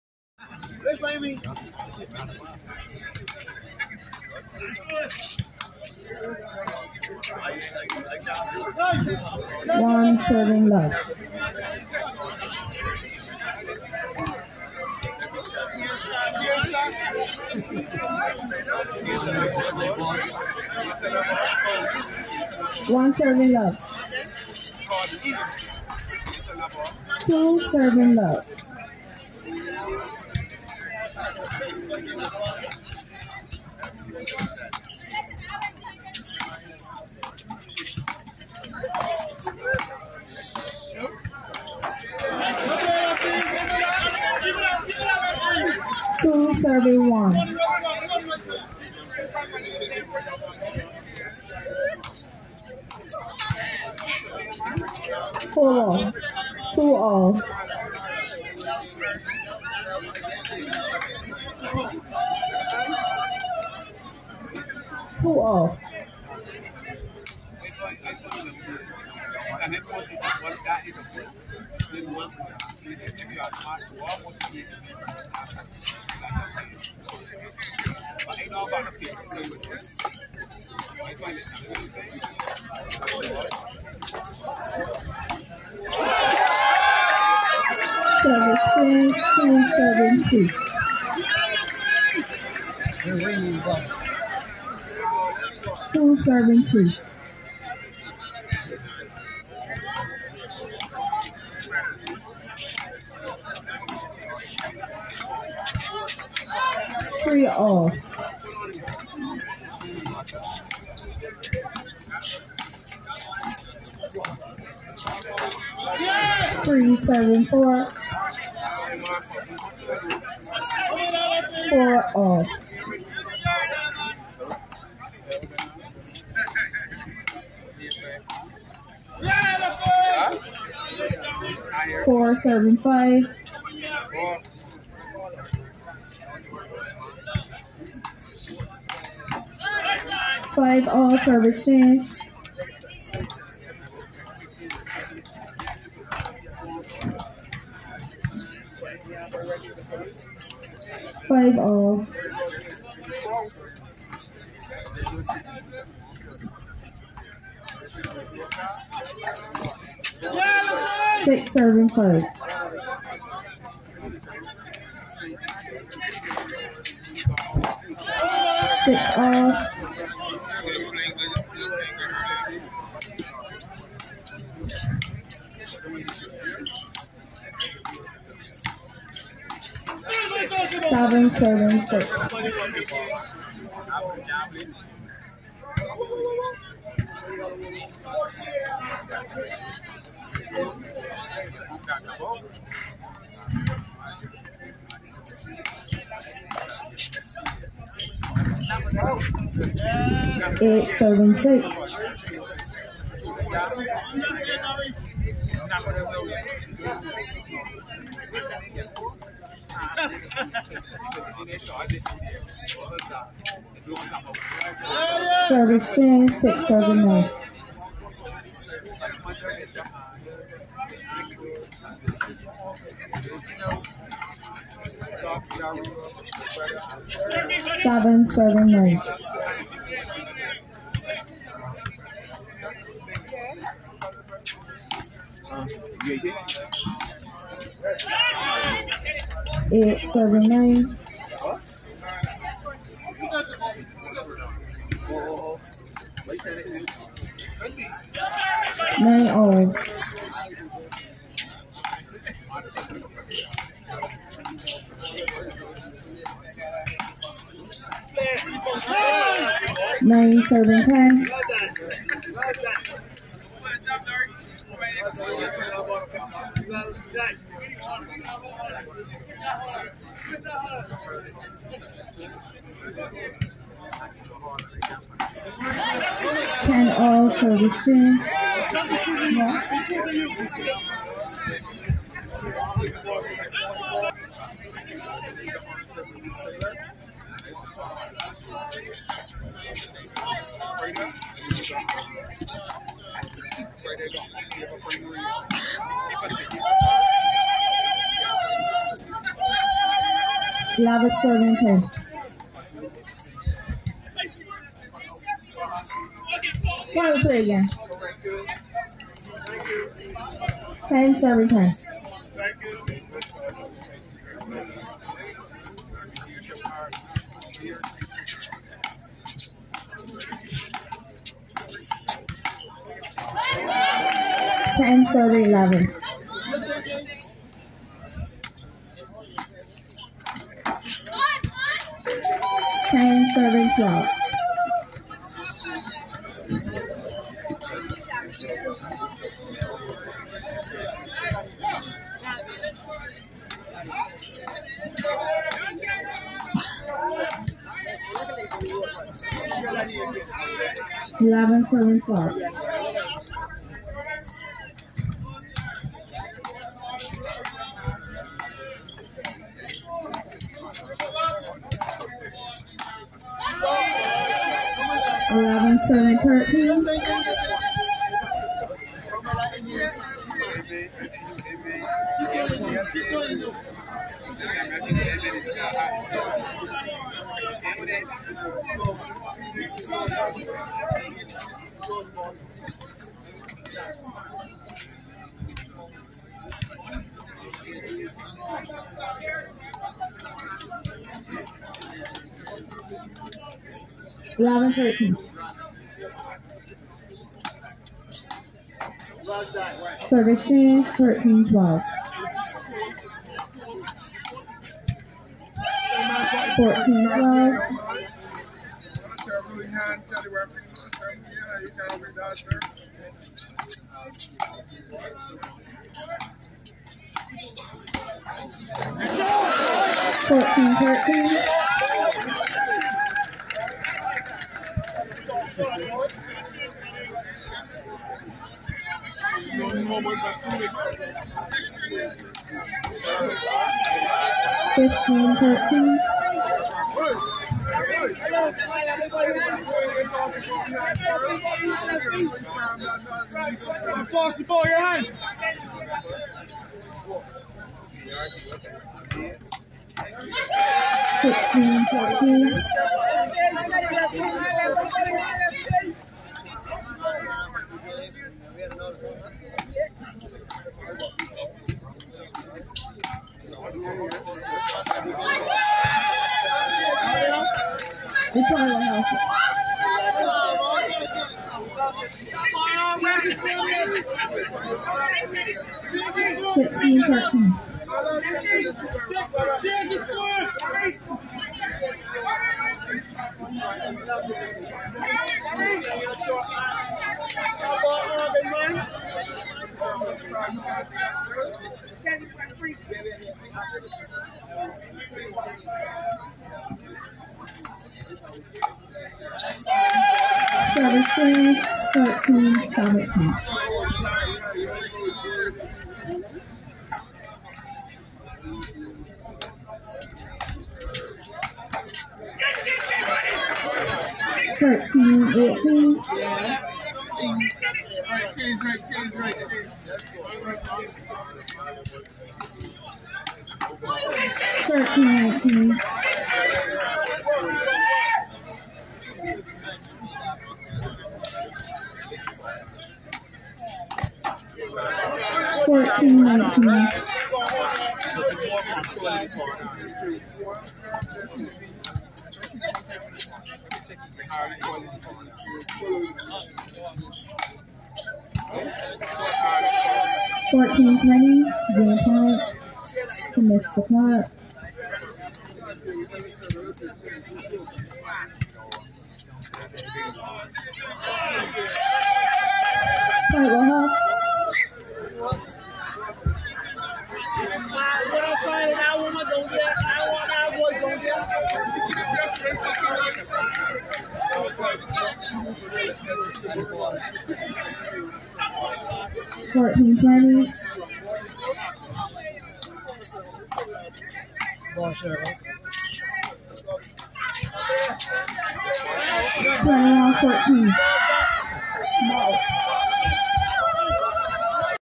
Hope for the Ghetto B Class road tennis tournament. [Sauve Clarke vs Darius Braithwaite)
Hope for the Ghetto. Round of 64